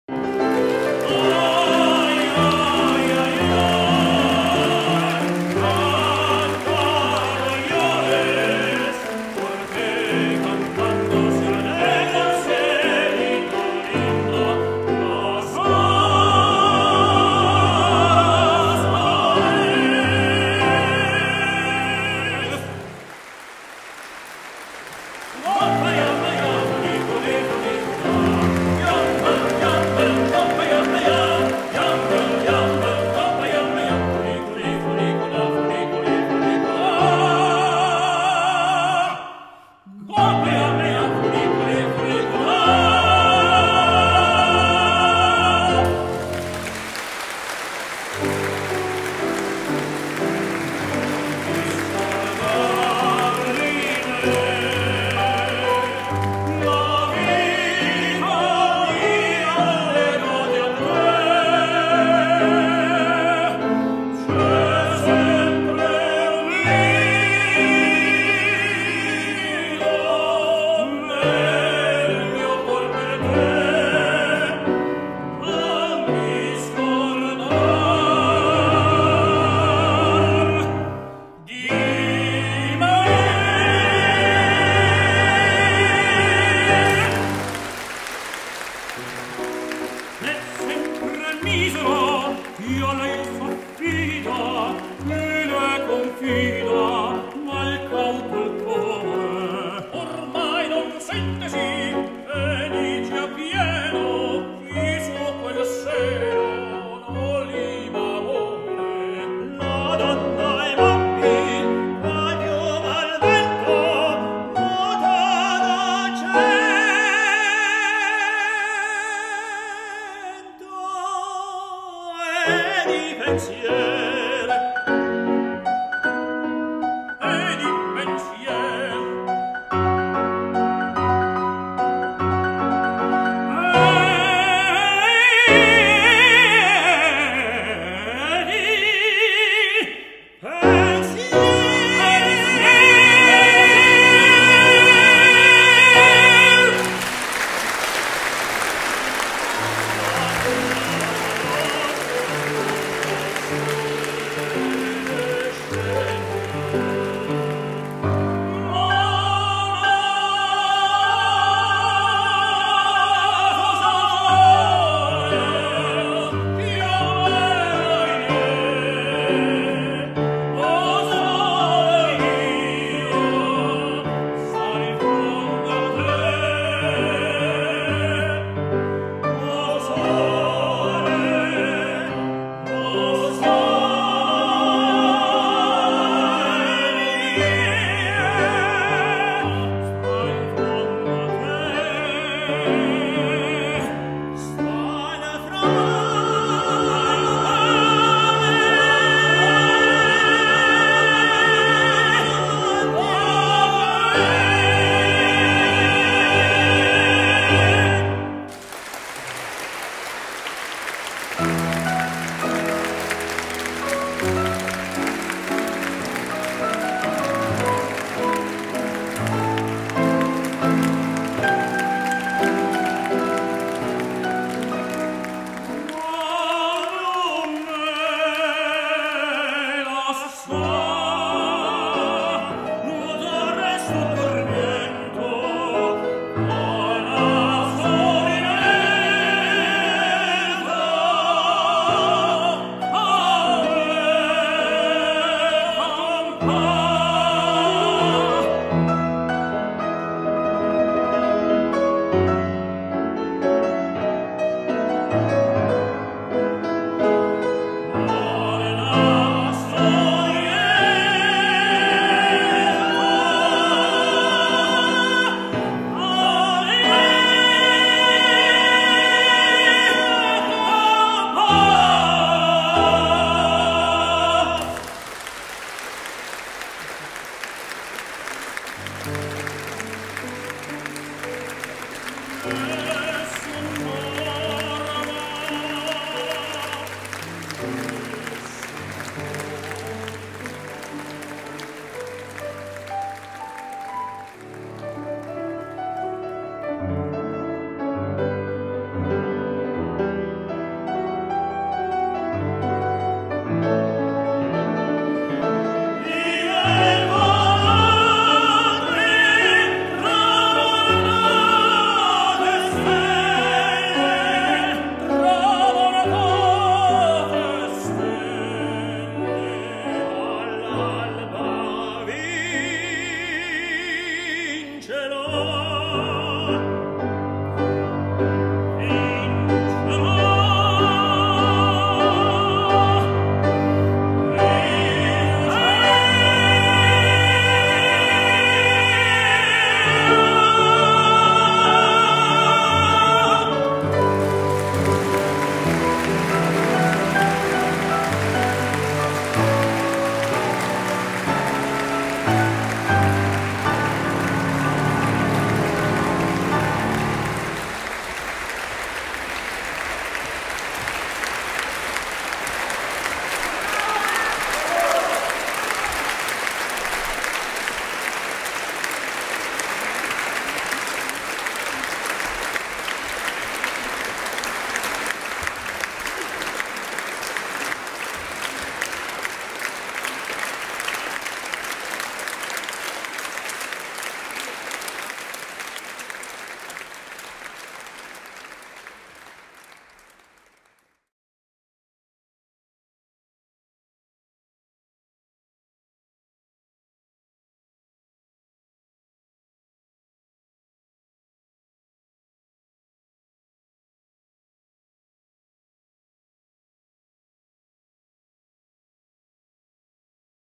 Three italian tenors - Video Demo
Three italian tenors: Antonio Sorrentino, Giampiero De Paoli, Antonello Mannarino - piano: Masha Kirillova - video demo recorded in Genova 2010